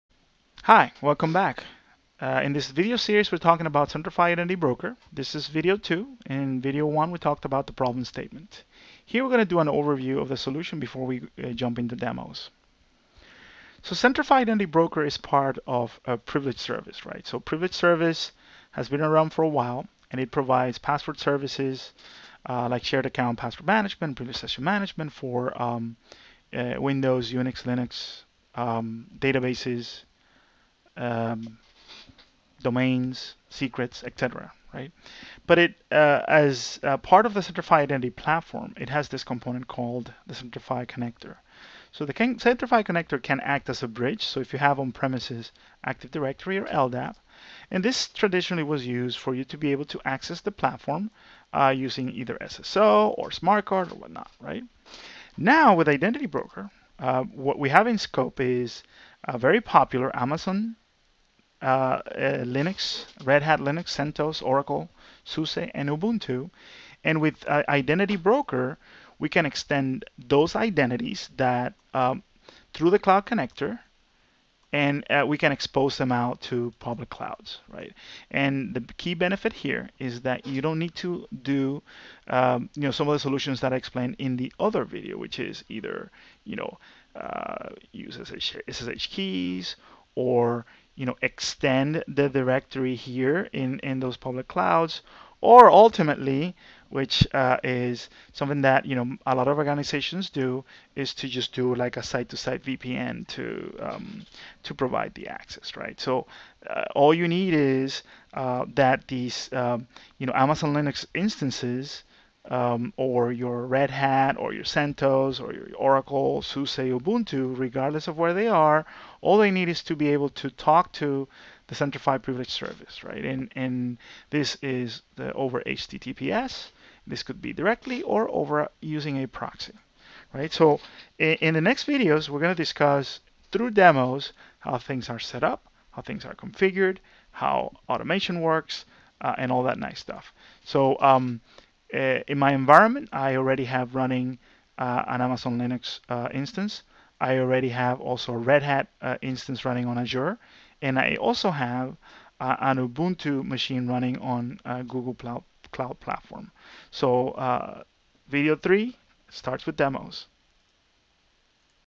Centrify Identity Broker - Solution Overview (2/5)
This video discusses how Centrify Privilege Service can help organizations bridge identity and extend to Linux workloads in any public cloud.